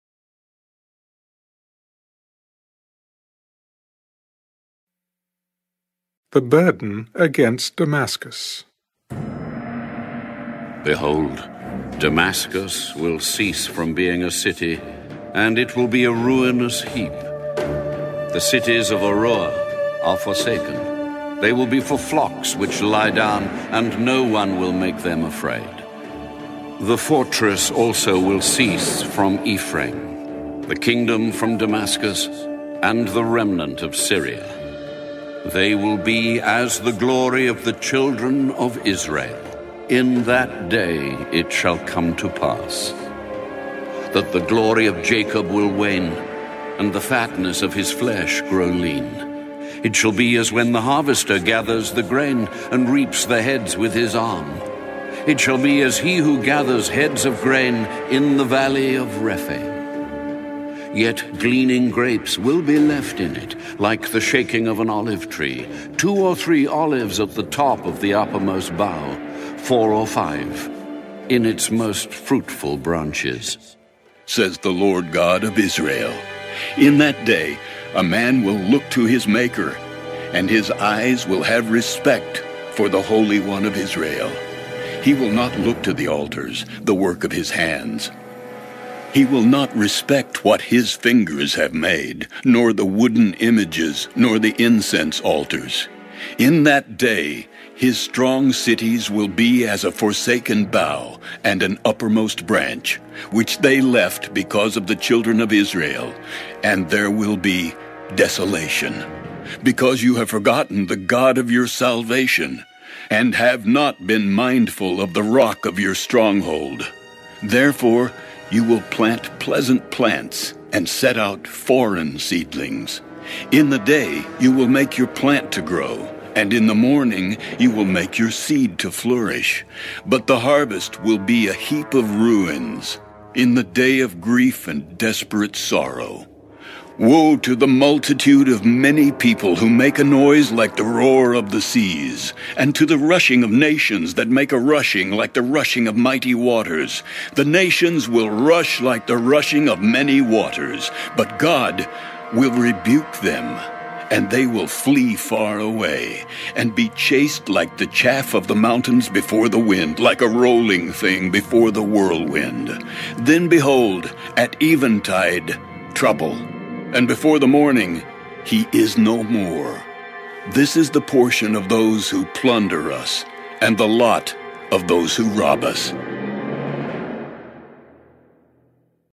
Isaiah 17 - NKJV (Audio Bible & Text)
Isaiah 17 - NKJV | Audio Bible & Read Along Text.

Sometimes this 23rd book of The Bible is known as "Isaiah", and sometimes it is known as "The Book of Isaiah". In this video you can listen to Isaiah 17 Audio Bible.